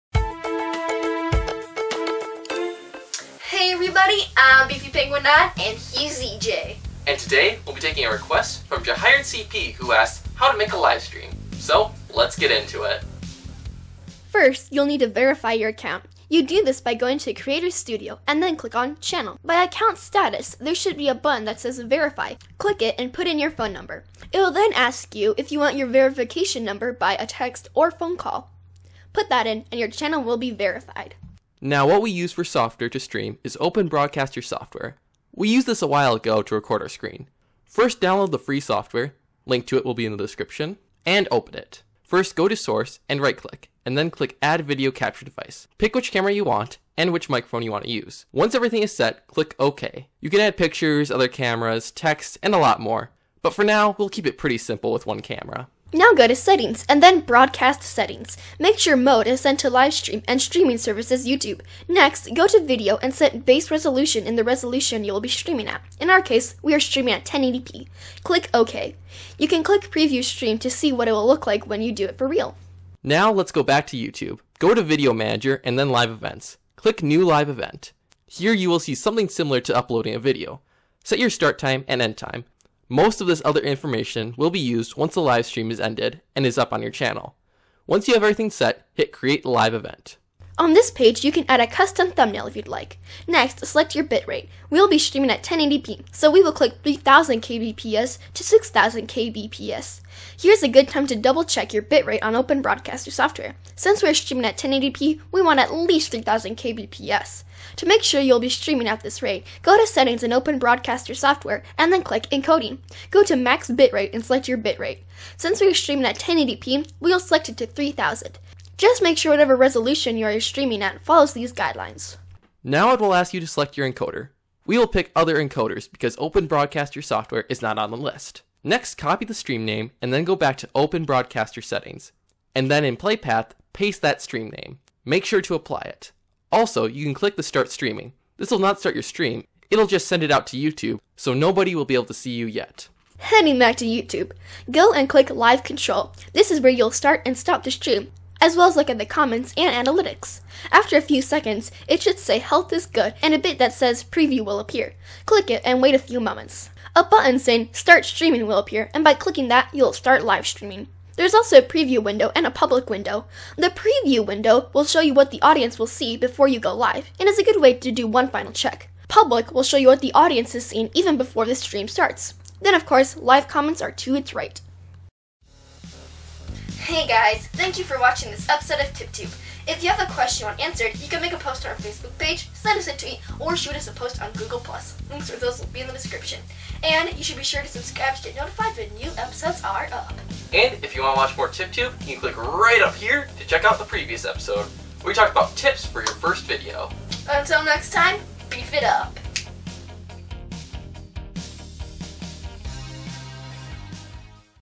How to Make a Live Stream on YouTube - TipTube Episode 8
Beefy and Z Jay show you how to do your very own live stream on YouTube.

Step 1: Verify your account. Do this by going to 'Creator Studio' and clicking on 'Channel.'

Step 2: Click the 'Verify' button.

Step 3: Put in your phone number

Step 4: Download 'Open Broadcaster Software' if you haven't already.

Step 5: Once the software is opened, go to 'Sources.'

Step 6: Right click 'Sources' and click 'Add'.

Step 7: A list of options will appear. Click on the one that says 'Video
Capture Device'.

Step 8: Choose which camera you will be using by going to 'Device'.

Step 9: Choose which microphone you will be using by going to 'Audio Input Device'.

Step 10: Once everything is set, click 'OK'.

Step 11: Go to 'Settings'.

Step 12: Click on 'Broadcast Settings'.

Step 13: Make sure 'Mode' is set to live stream, and that 'Streaming
Service' is on YouTube.

Step 14: Go to 'Video'.

Step 15: Set the resolution in which you will be streaming at.

Step 16: Click 'OK'.

Step 17: Open up YouTube and go to 'Video Manager'.

Step 18: Click 'Live Events'.

Step 19: Click 'New Live Event'.

Step 20: Set your start time and end time.

Step 21: Once you have everything set, click 'Create Event'.

Step 22: Select your bit-rate.

Step 23: Go back over to 'Open Broadcaster Software'.

Step 24: Click on 'Settings'.

Step 25: Click on 'Encoding'.

Step 26: Go to 'Max Bit-rate' and make sure that your bit-rate is the same to the one on YouTube.

Step 27: Head back over to YouTube.

Step 28: Find where the page says 'Select Your Encoder'.

Step 29: Click 'Other encoders'.

Step 30: Copy the stream name and go to the settings page on 'Open
Broadcaster Software'.

Step 31: On the 'Play Path/Stream Key' setting, paste your stream name.

Step 32: On the front page of 'Open Broadcaster Software' click the 'Start Steaming' button.

Step 33: Head back to YouTube.

Step 34: Click on 'Live Control Room'.

Step 35: Click the button that says 'Preview'.

Step 36: A button that says 'Start Streaming' will appear. Click that button and your live stream will begin!